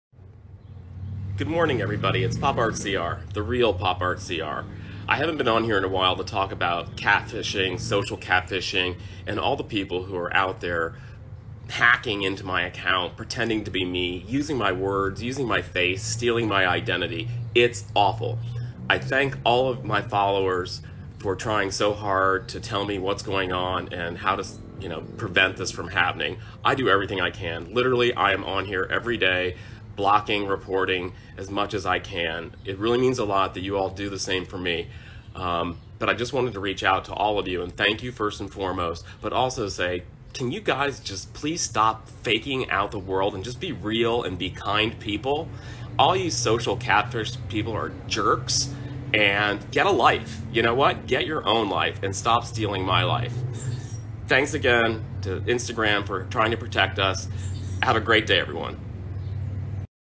Chris Obetz needs to speak again
Christopher Obetz
NEEDS TO SPEAK OUT
Again.

Criminals are using HIS IMAGES everywhere for Romance Fraud...

KNOW CHRISTOPHER IS NOT TALKING TO ANYONE AND HE DOES NOT LIKE THIS.. HE CAN DO NOTHING ABOUT IT.